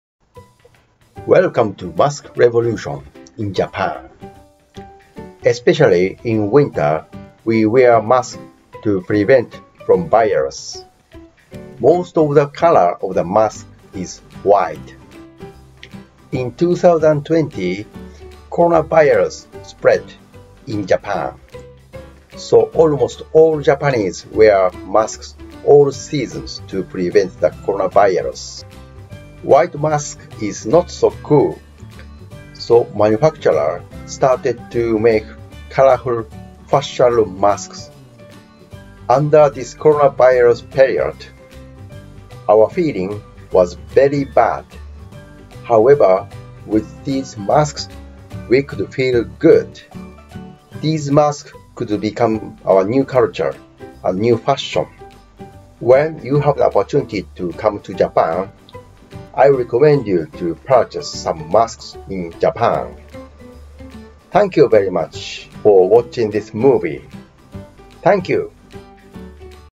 Mask Revolution in Japan
Japanese wear only white masks before corona virus.
However, colorful / fashionable mask has been getting popular in 2020.